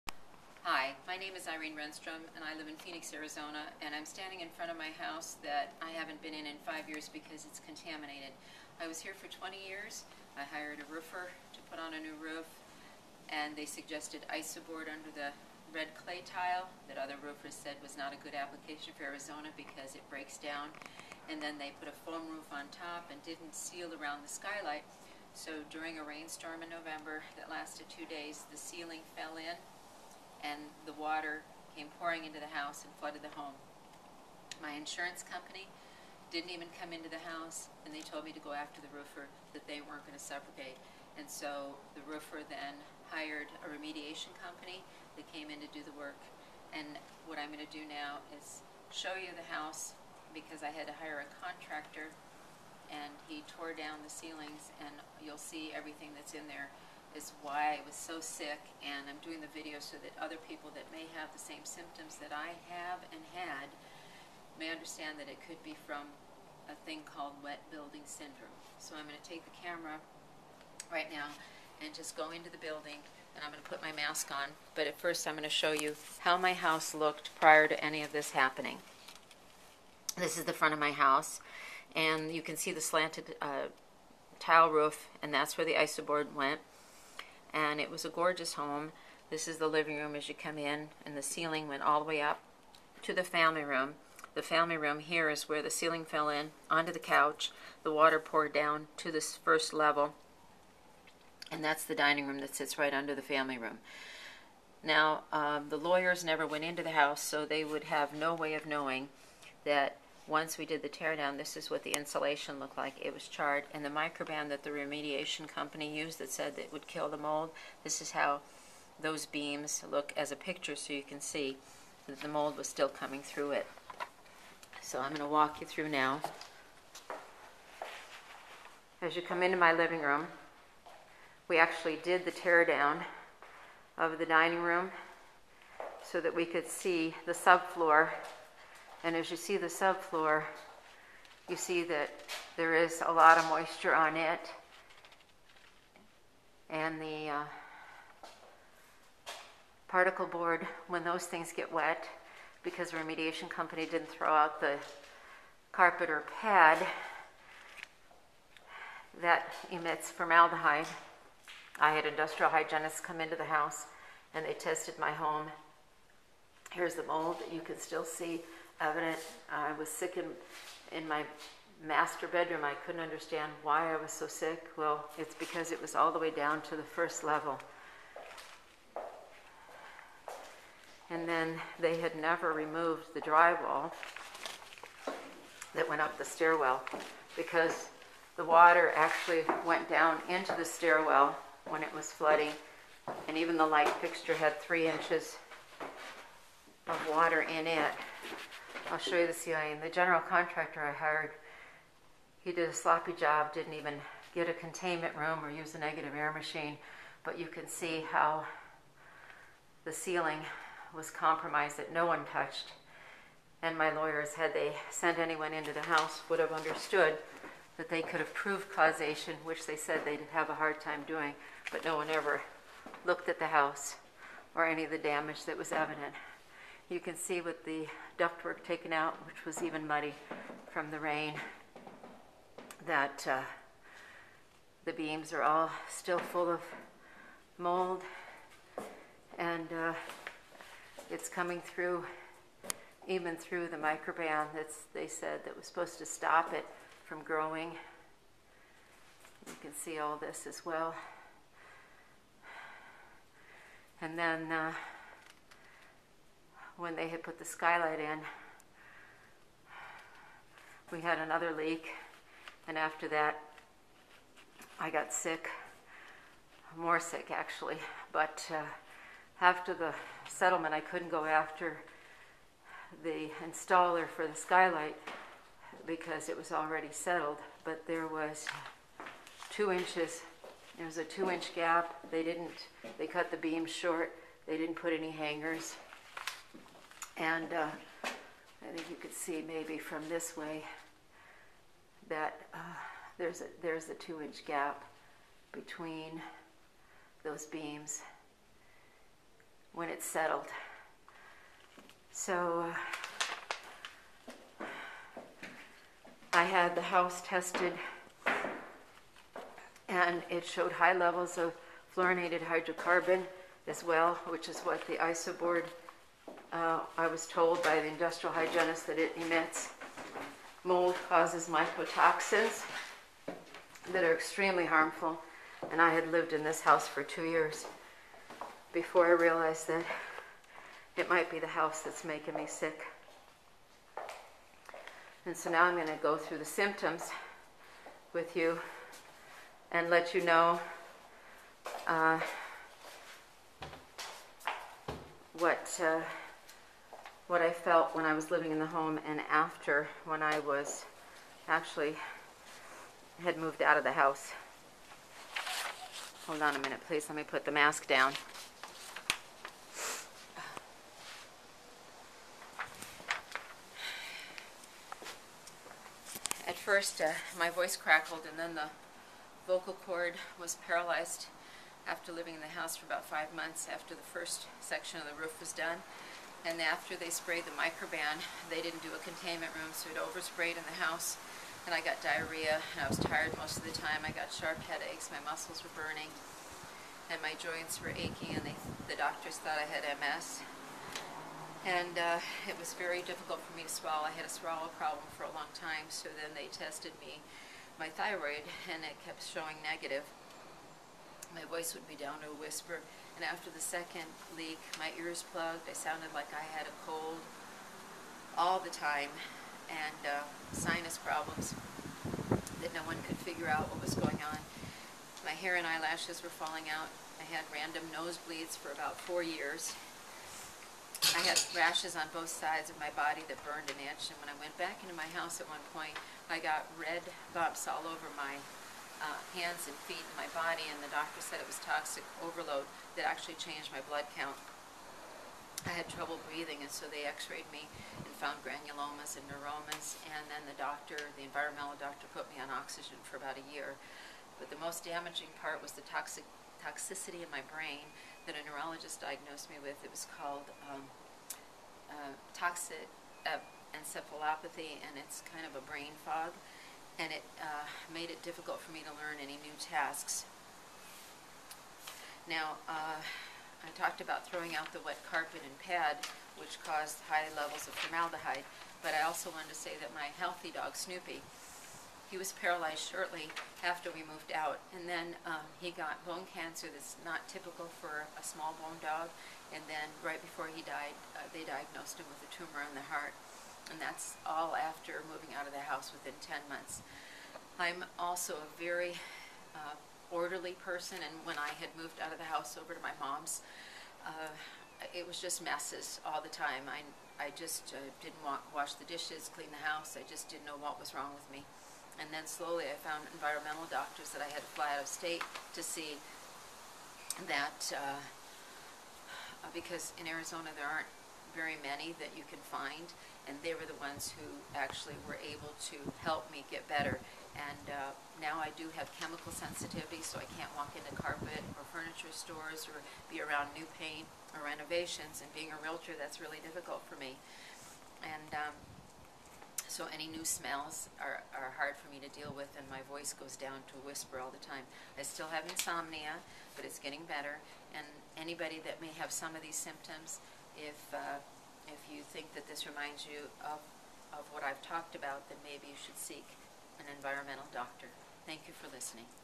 Healthy Dog and Owner Get Sick From Toxic Mold House
I had a roofer put on isoboard and a foam roof on a home I lived in for 20 years. Did not seal around skylight and water came in for 2 days during a rainstorm at the end of the year and ceiling collapsed. Roofer hired restoration company that did not do containment room and over sprayed Microban throughout the house but the pictures show that mold grew through microban as well.

I got very ill living in the house and I describe symptoms in video. Paralyzed vocal chord, swallow problems, sharp headaches, lethargy, diarrhea , achy joints and muscles burned, black vertical lines on my nails, swallow problems, hair and eyelash loss, bloody noses, rashes on both sides of my body, red water blister bumps that mimicked leukopenia in changing WBC, brain fog diagnosed as toxic encephalopathy making learning new tasks difficult as well as remembering learned tasks. Mycotoxins from mold have been thought to affect the brain. I couldn't tolerate bright lights or loud noises and my nails had dark vertical ridges as well.

The Formaldehyde affects my vocal chord so that even now walking into a furniture/carpet store makes my voice goes to a whisper. The same happens with new glue down carpet or renovations or exposure to many colognes or scents.  Insomnia has gotten somewhat better.  I had fullness in my ears that mimicked blocked ears and I sounded like I had a cold for a whole year until I moved out of the house.

My shzi-tsu, Snoopy, was healthy and then became paralyzed as soon as we left the house then came down with a limp and had bone cancer in his leg. Ten months later he had a tumor on his heart and died on operating table.

Environmental doctors tested me for mycotoxins and formaldehyde exposure and found I was reactive to most harmful molds and extreme reactive to formaldehyde that is found in Wet Buildings that is referred to as "Sick Building Syndrome"

I went through a series of IV's to detox as well as Infar Red Saunas with nutrients for over two years and I was put on Oxygen as I had low blood gas. Lungs had granulomas and nodules and I was slightly asthmatic before this happened and I still have breathing issues. Thyroid tested for swallow problems as well.

I am feeling better being out of the house but I need special paint on walls, tile floors, non-formaldehyde flooring and insulation and many special items that a woman called "The House Doctor" told me to use so that I could live in a renovated home.  Off gassing is a must for me as I can't walk into newly renovated homes with basic products.

Decon7 was used by the general contractor but it did not kill the mold as it was still evident and needed to be sanded from the beams. I still need a mask when I go into the home and hope someone can help me do the correct put back.